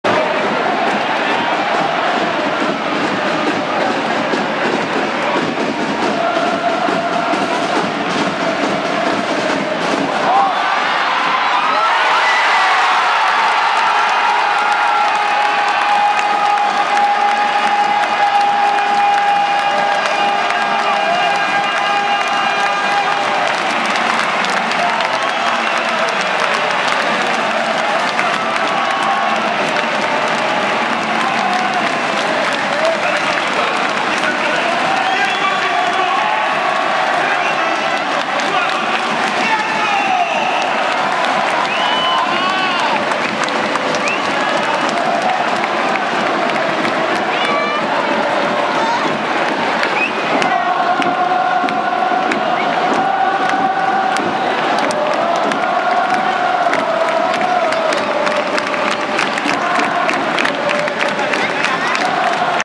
2016.10.1 ヴィッセル神戸 vs 川崎フロンターレ レアンドロの2点目
ダメ押しゴール。ポケモンみたいな光が出ますが、ノエスタのライトの映り込みです。けど、後光が射してもいい活躍、得点王。